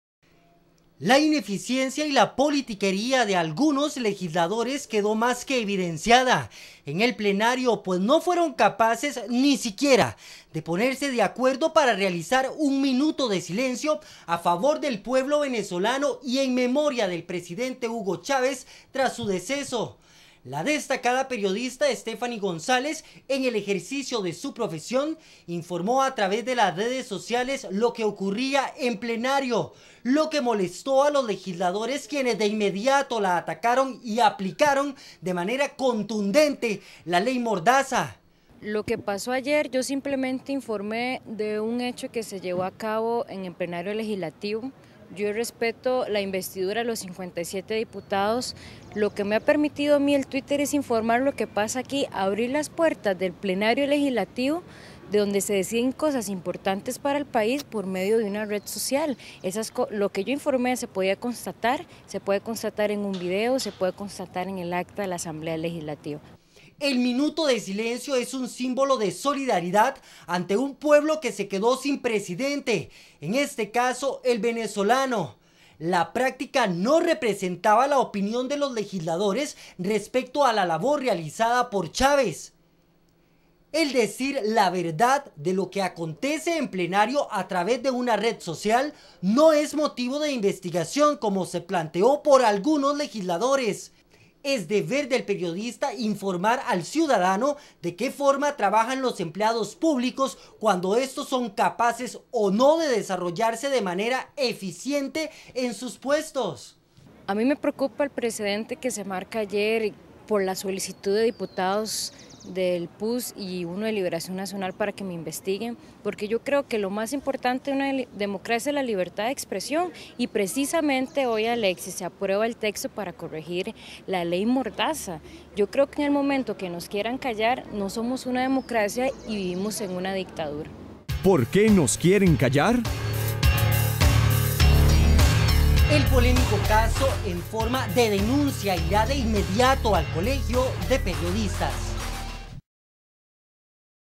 LEY MORDAZA 1
ESTO SUCEDIO EN NUESTRA ASAMBLEA LEGISLATIVA, VEA Y SAQUE SUS PROPIAS CONCLUCIONES Y OPINES